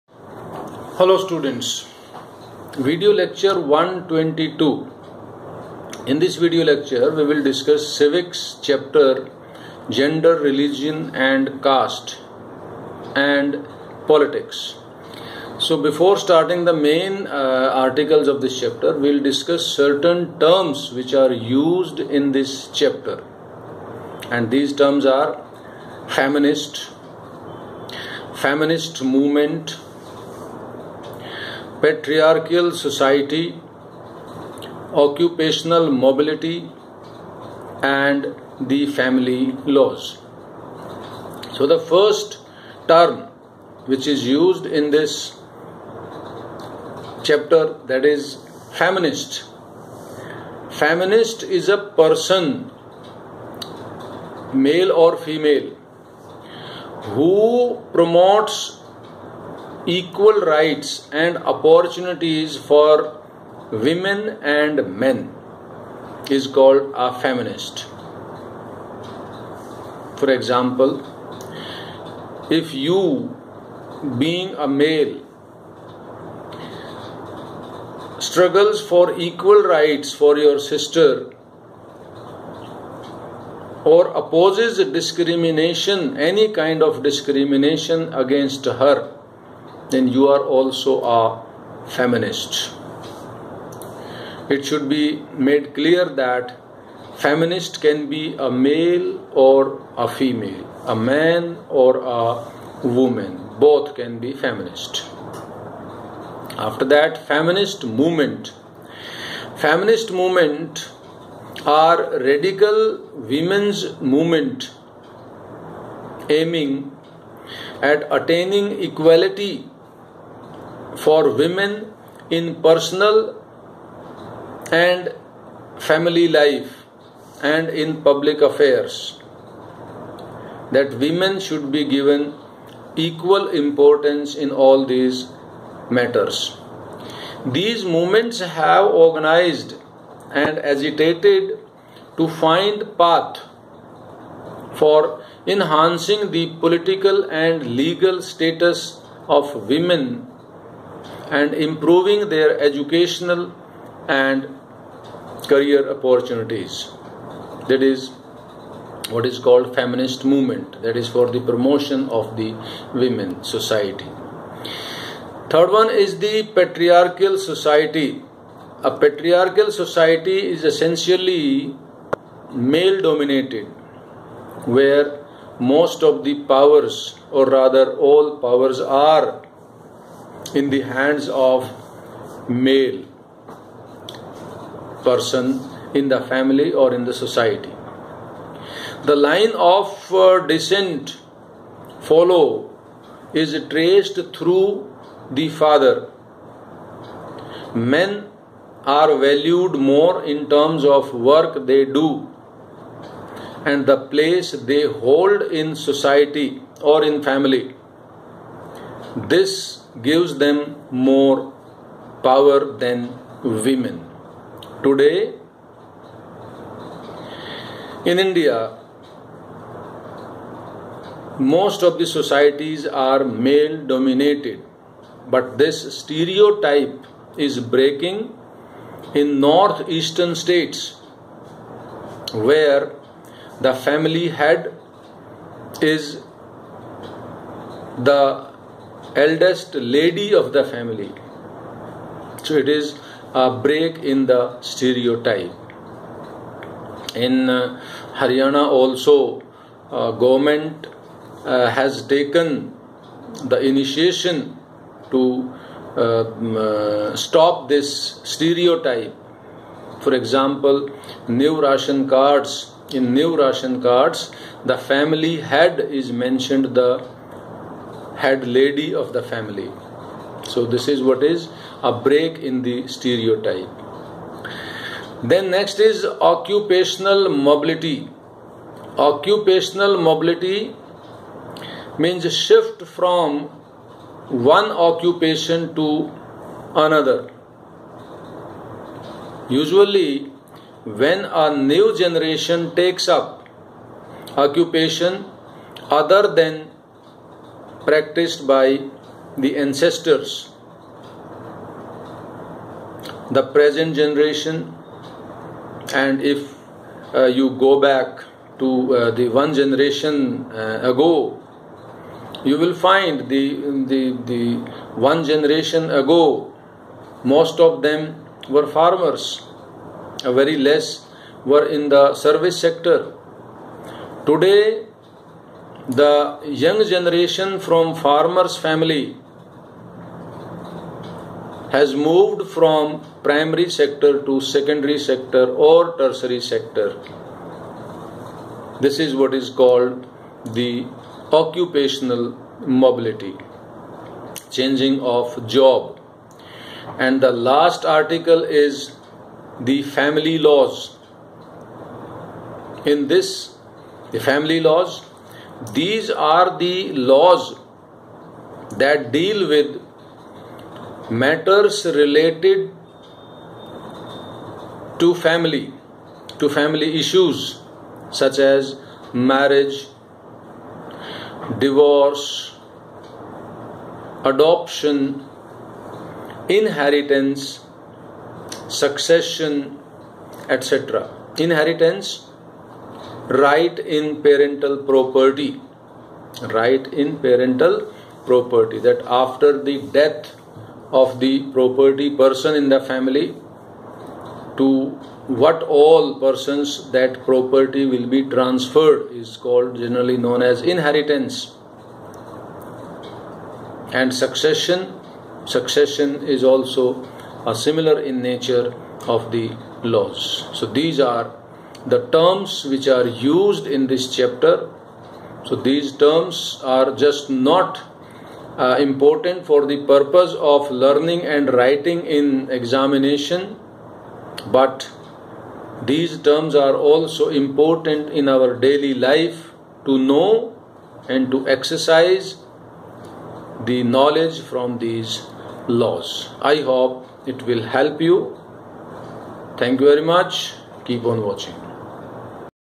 Lecture 122 by DS Jakhar based on Civics chapter Gender , Religion and Caste.
1. Important terms used in this lecture are as follows.

  a. Feminist meaning and Feminist Movements.

  b. Patriarchal Society.

  c. Occupational mobility.

  d. Family laws.

Note:- These terms are generally asked in examination as objective type questions.

.thanks.